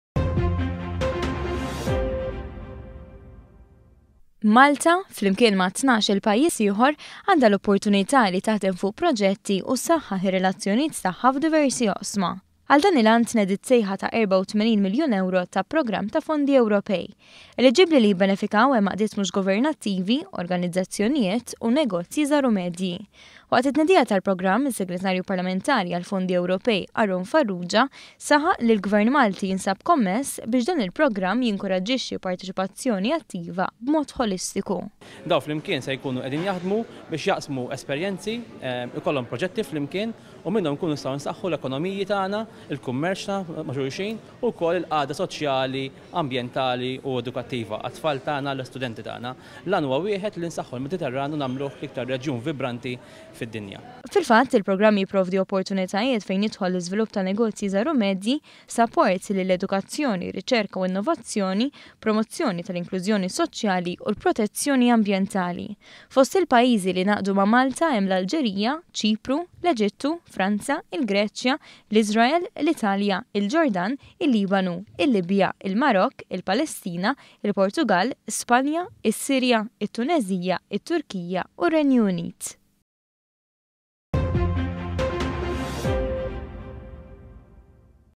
PROGRAMM TA’ €84 MILJUN MINN FONDI EWROPEJ.
ONE.COM.MT • Imniedi programm ta’ erba u tmenin miljun ewro ta’ fondi Ewropej. Programm li Malta flimkien ma’ tnax-il pajjiż ieħor ser isaħħaħ ir-relazzjonijiet tagħha fuq diversi oqsma fosthom l-iżvilupp tan-negozji żgħar u medji, sapport fl-edukazzjoni, ir-riċerka u l-innovazzjoni, promozzjoni tal-inklużjoni soċjali u protezzjoni ambjentali.